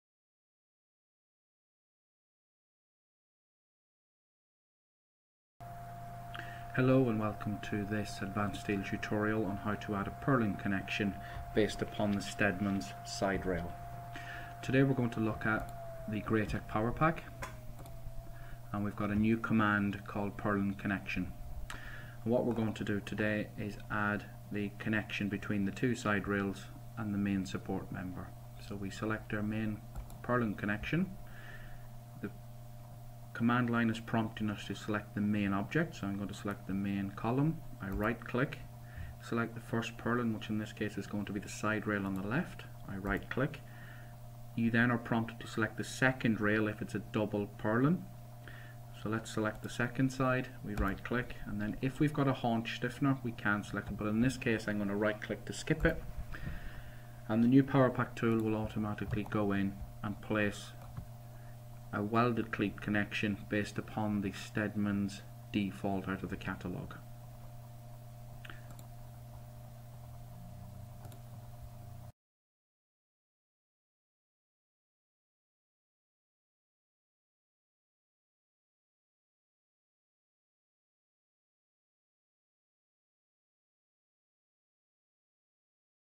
Advance Steel 2017 – Adding Steadmans Welded Cleat Connection
Learn how to add a Steadmans Welded Cleat Connection in Advance Steel 2017 with the Graitec PowerPack.